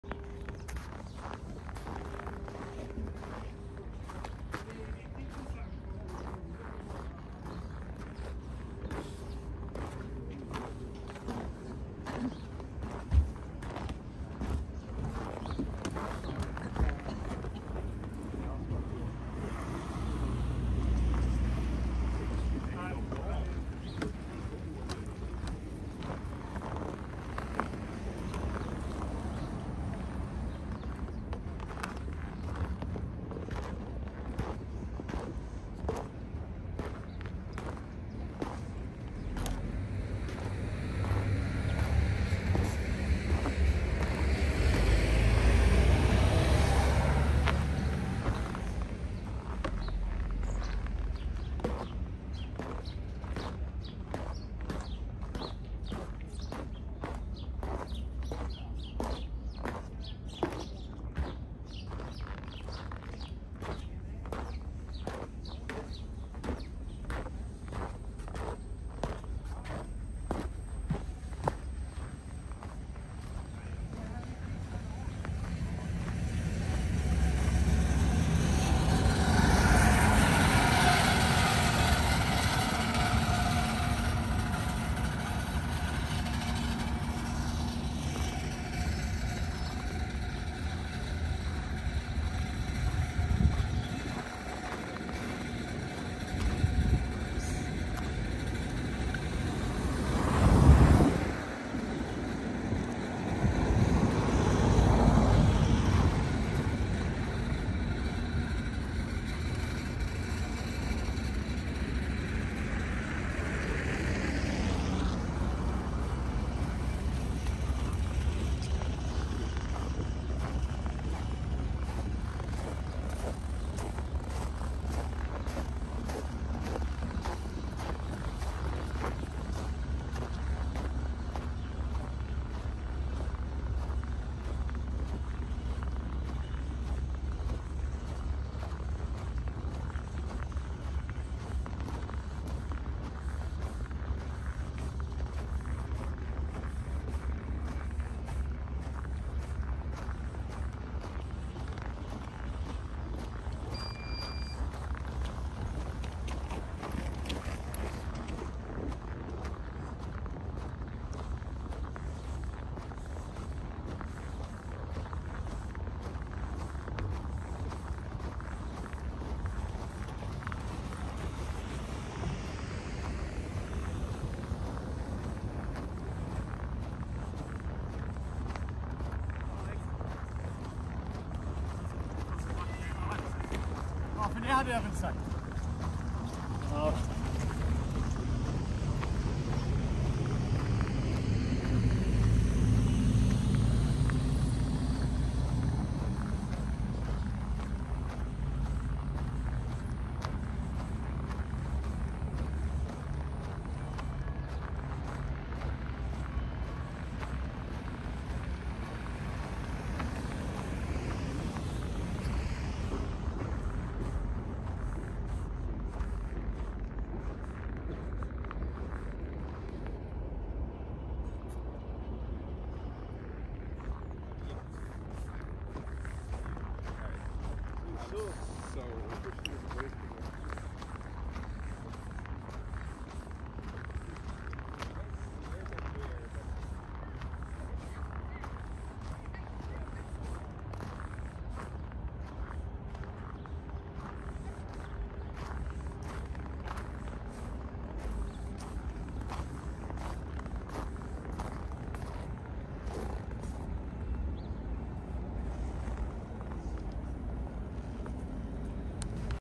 Sweden 🇸🇪;Winter walk in Stockholm 2022
Beautiful view of Stockholm
Walking tour through Dihlströms trappor to Katarinavägen